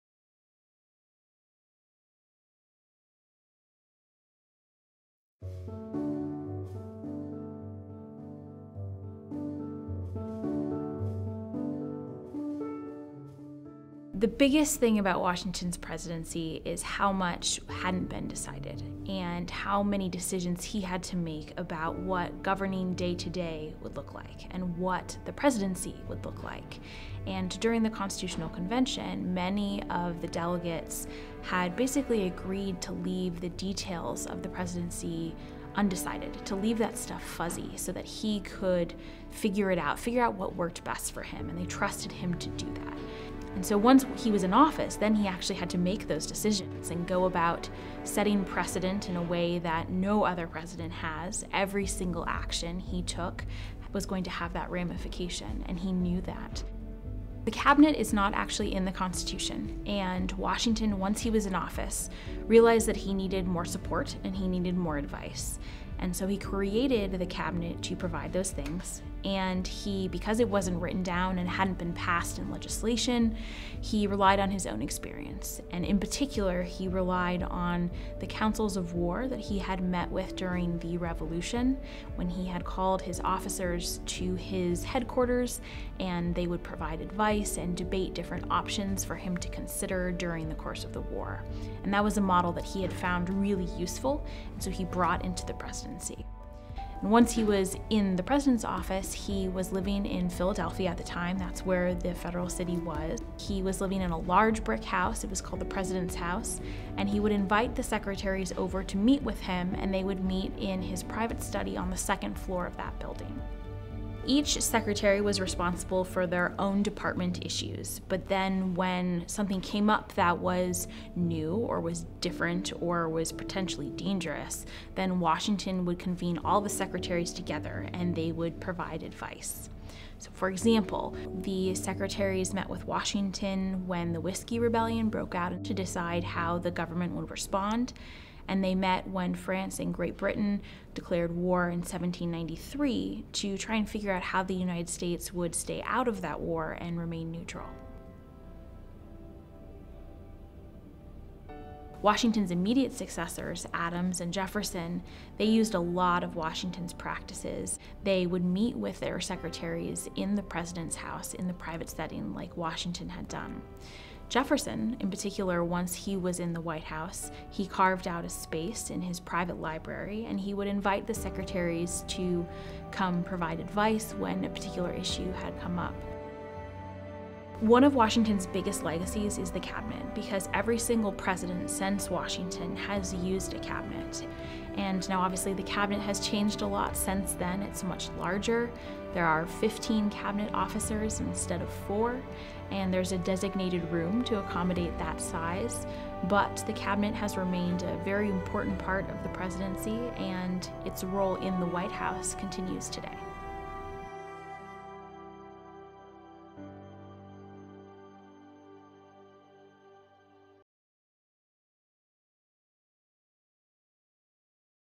The Origin of the President's Cabinet
Learn about George Washington's creation of the cabinet, an advisory group for the President of the United States of America, and the cabinet's place in White House history. Featuring Dr. Lindsay Chervinsky, historian at the White House Historical Association and author of "The Cabinet: George Washington and the Creation of an American Institution" (available April 2020).

Use this video as a supplement to the WHHA Classroom Resource Packets

For more short, educational videos from the White House Historical Association, please